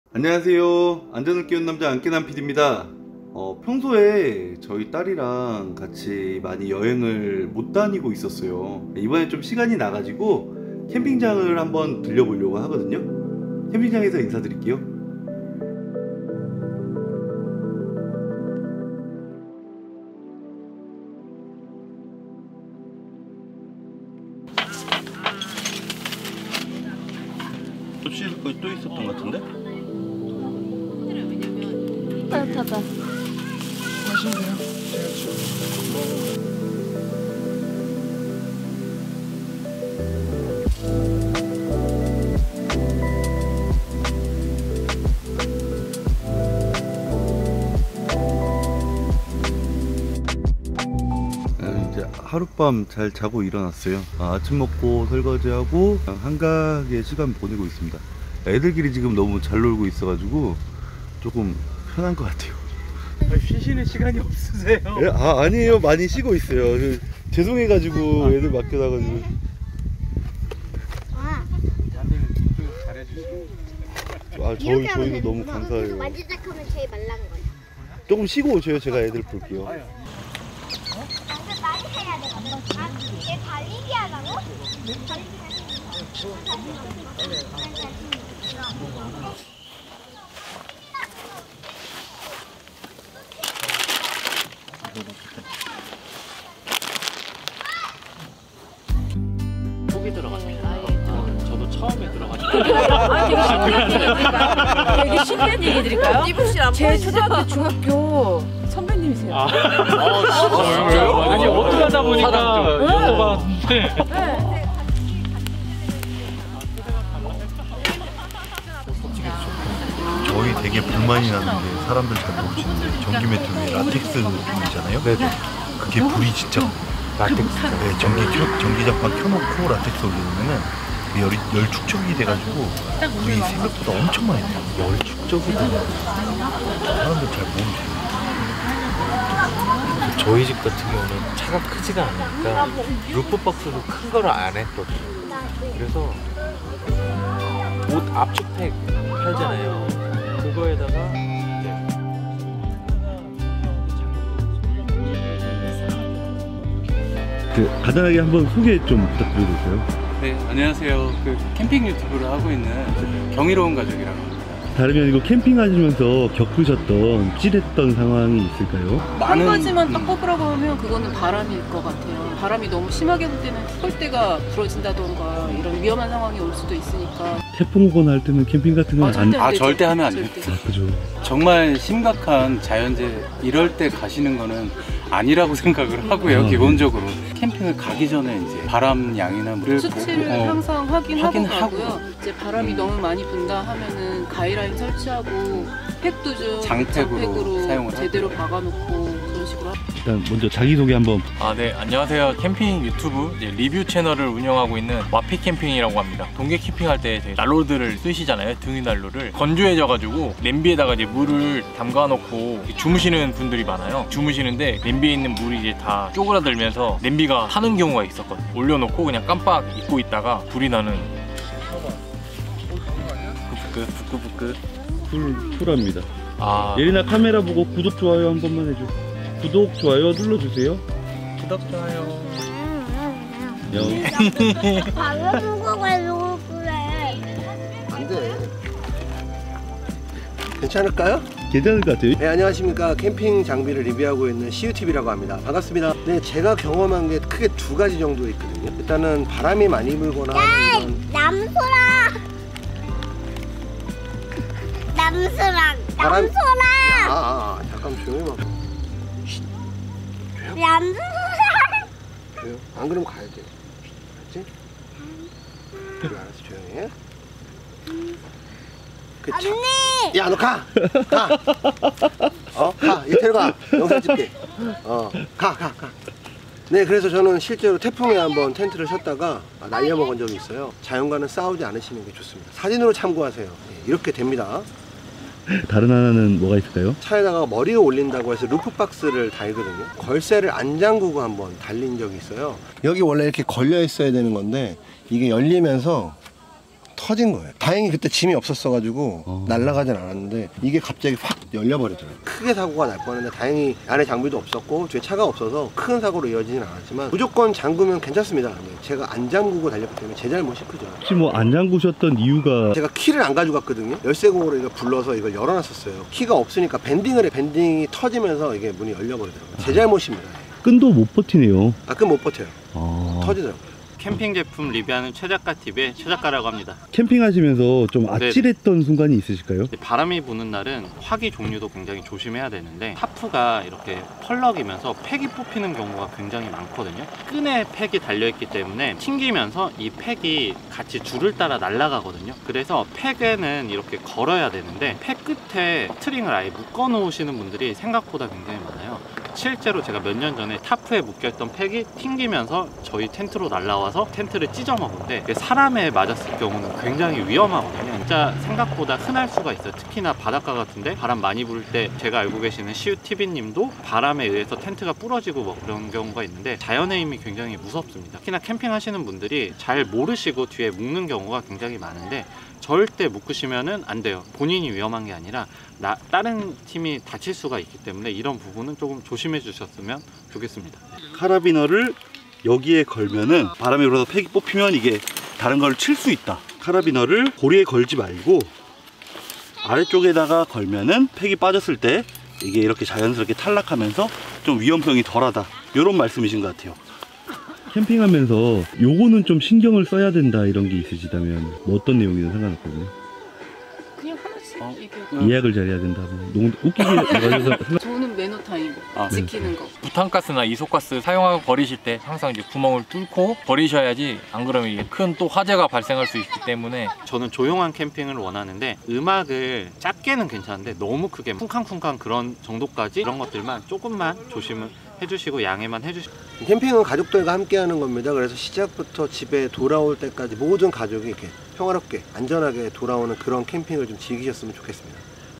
캠핑 유튜버들의 아찔한 사고 / 딸과 캠핑 VLOG / 🏕 유튜버들과 만남 마이캠프페스티벌
ㅡㅡㅡㅡㅡㅡㅡㅡㅡㅡㅡㅡㅡㅡㅡㅡㅡㅡㅡㅡㅡㅡㅡㅡㅡ

안녕하세요! 안깨남PD 김찬수입니다
캠핑을 즐기는 편인데 최근에는 너무 바빠서 가족과 여행을 많이 못갔어요.
연휴기간을 활용해 2박 3일 캠핑을 다녀왔답니다.

캠핑장에서 캠핑 경력 다수의 유튜버분들을 만나
안전한 캠핑을 위한 노하우를 전수 받았답니다

특히 마지막에 팩에 로프를 고정하는 방식은 저도 전혀 생각을 못했던 부분이네요 세잎이께도 유용한 정보가 될 것 같아요

캠핑을 즐기시는 모든 분들 늘 안전에 유의하시고 행복한 시간 보내셨으면 합니다.

우정출연 : @wonderful_family  @yp_camping  @CamperJagga  @CU_TV